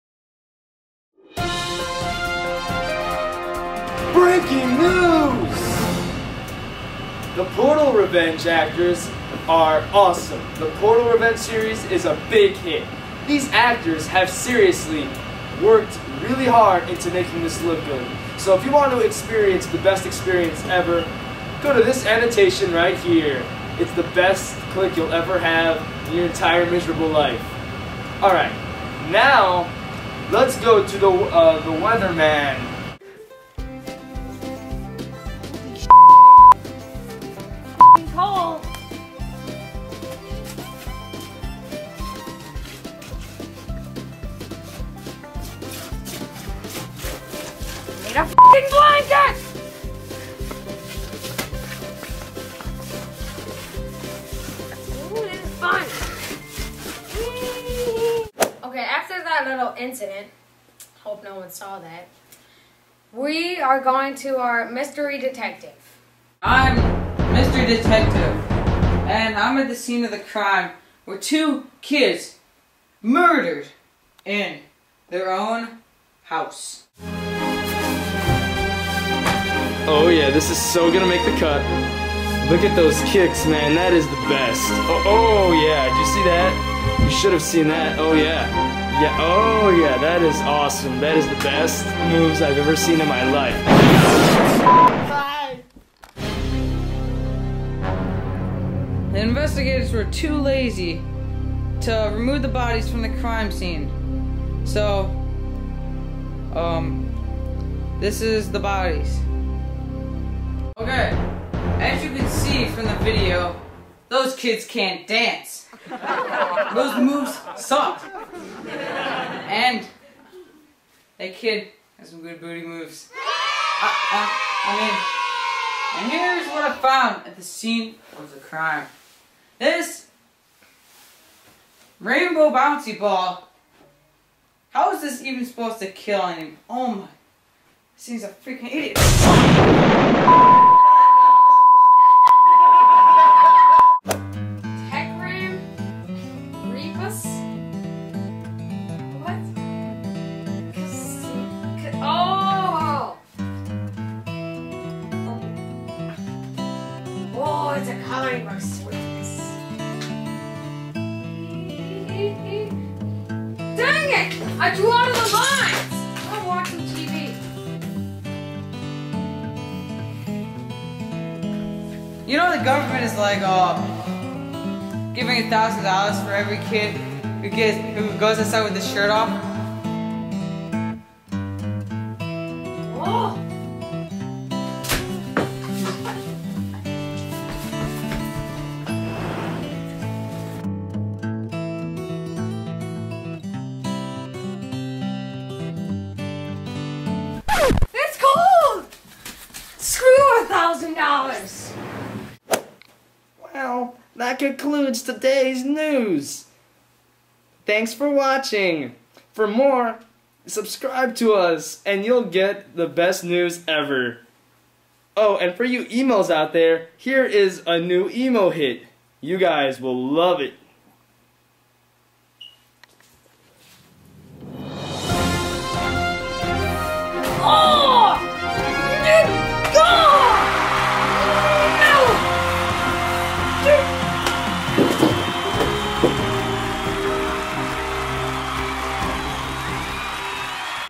The Fingathing News #1
We give out the best news ever. What will happen with the rainbow ball murder case? After mystery detective gets out of the hospital we'll find out.